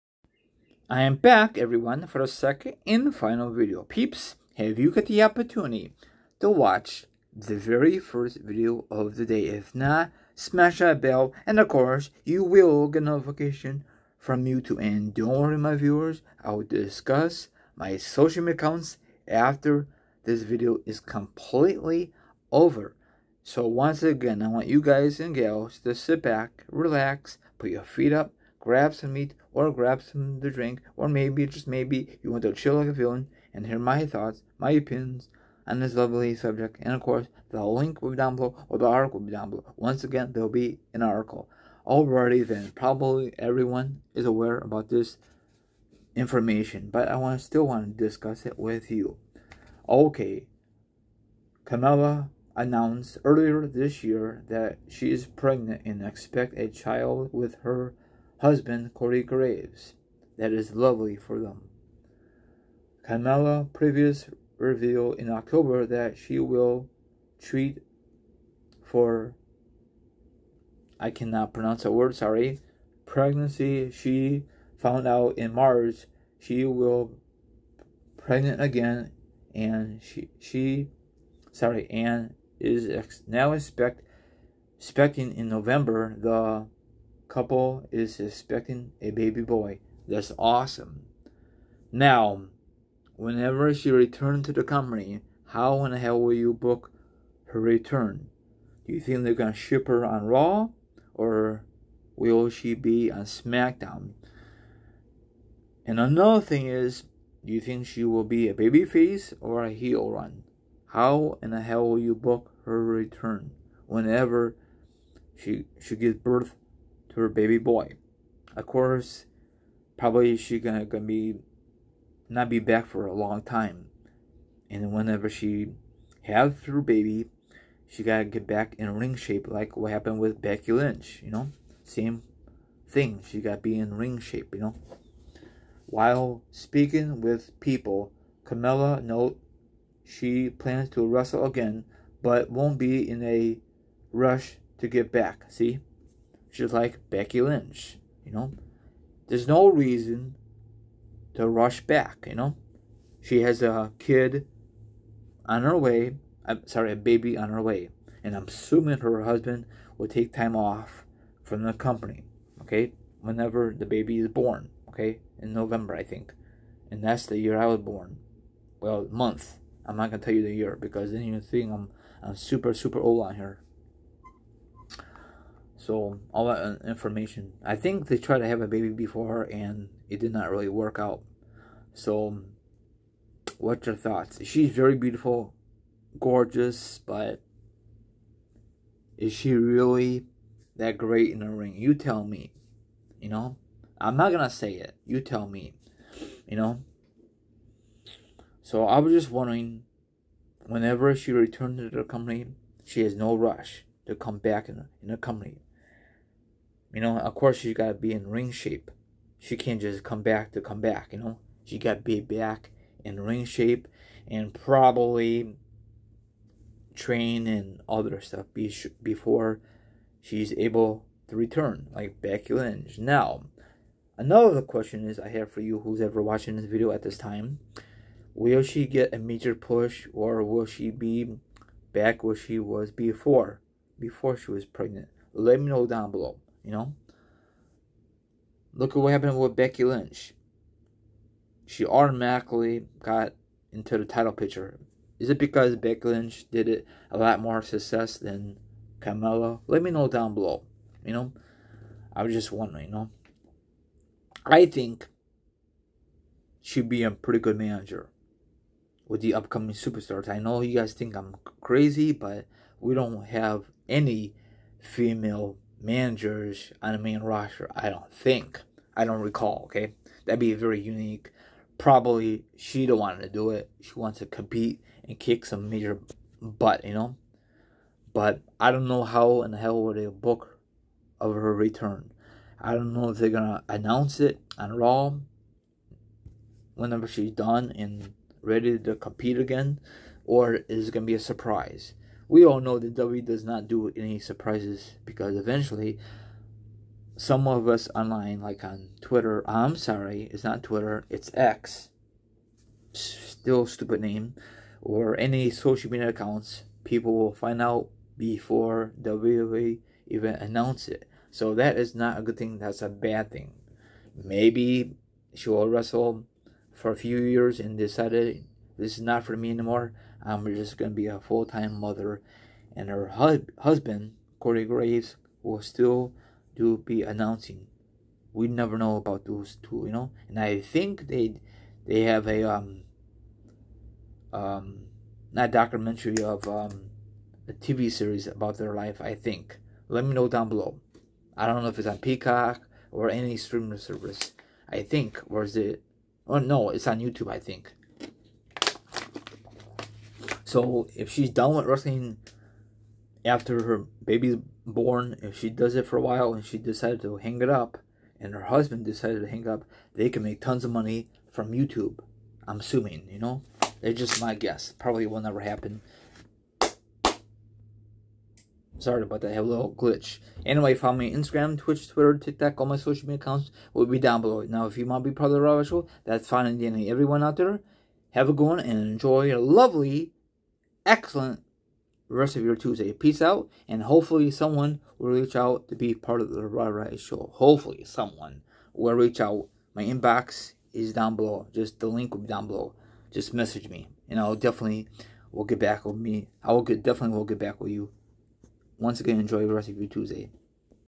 Carmella Plans On Returning To WWE After Giving Birth
Carmella announced earlier this year that she is pregnant and expecting a child with her husband, Corey Graves.

Carmella previously revealed in October that she was treated for an ectopic pregnancy. She found out in March that she was pregnant again and is now expecting in November. The couple is expecting a baby boy.

While speaking with PEOPLE, Carmella noted she plans to wrestle again but won’t be in a rush to get back.

“I for sure plan to come back. I think it’s important to show you can have a baby and still have a career, especially a very physical one at that, but I’m not going to rush,” she said. “I really just want to take my time recovering and making sure that I feel great and ready to go back and not sort of rushing because I don’t even know what to expect as far as how I’m going to feel after having a baby.